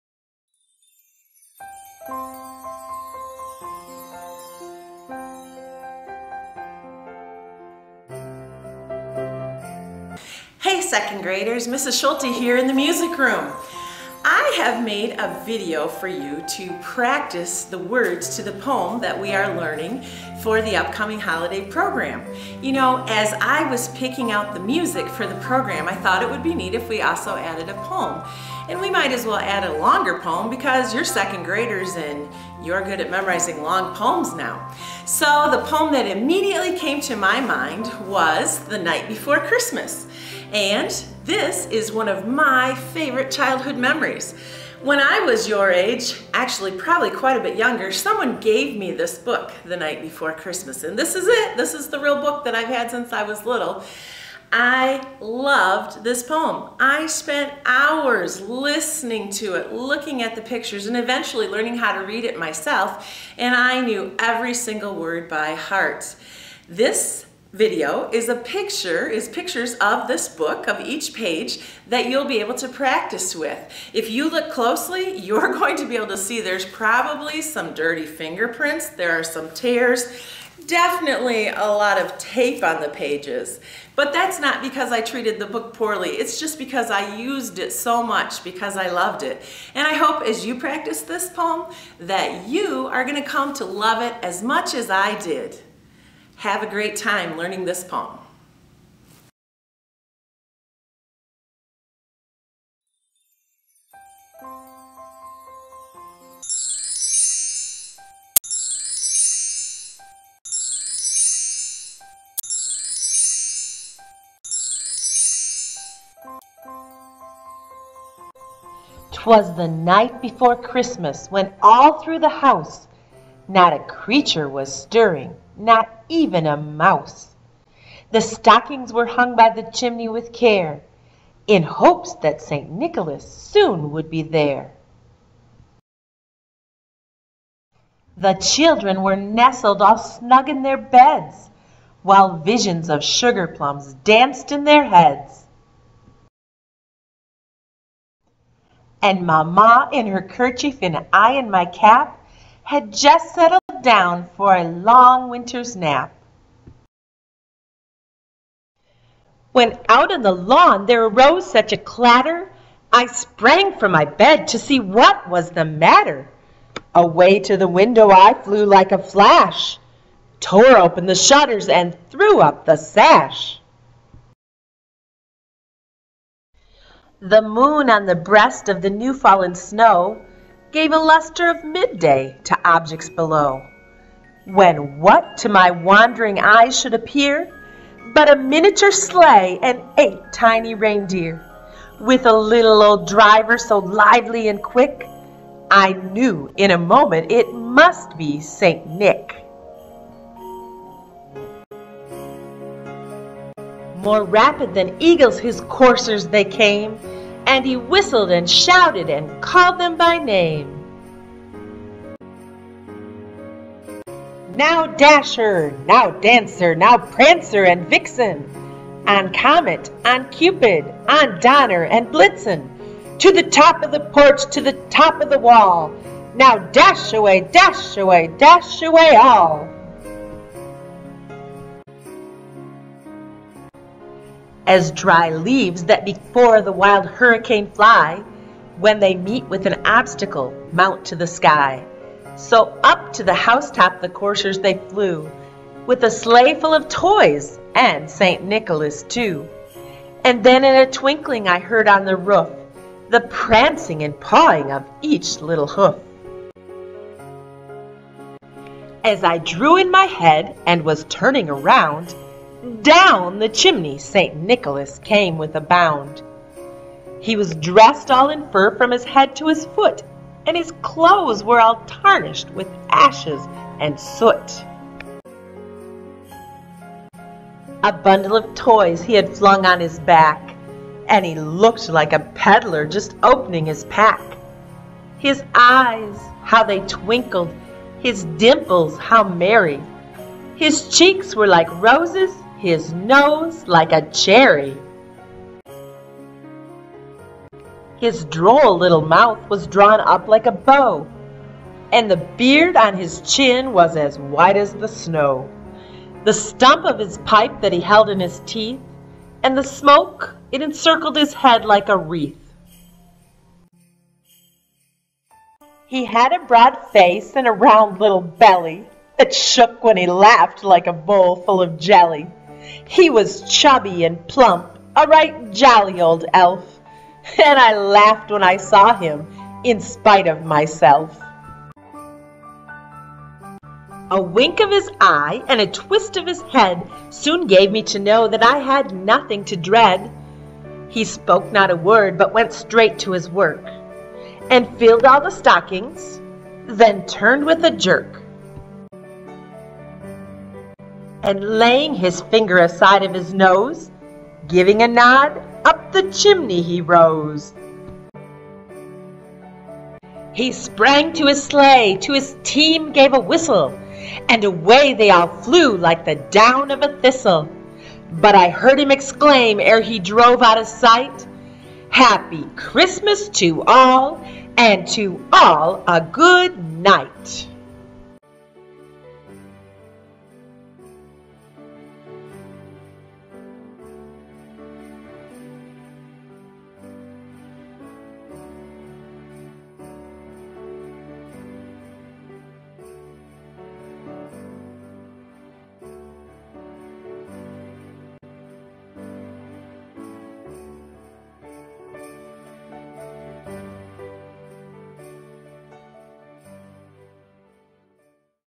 'Twas the Night Before Christmas Book- Clement C. Moore
Mrs. Scholte reads "The Night Before Christmas" written by Clement C. Moore for her students. She begins the video describing that the poem (and this particular book illustrated by Catherine Barnes 1960) was one of her childhood favorites. Poem begins at 1:50. This video was created for students to practice reading and memorizing the lines of the beloved poem.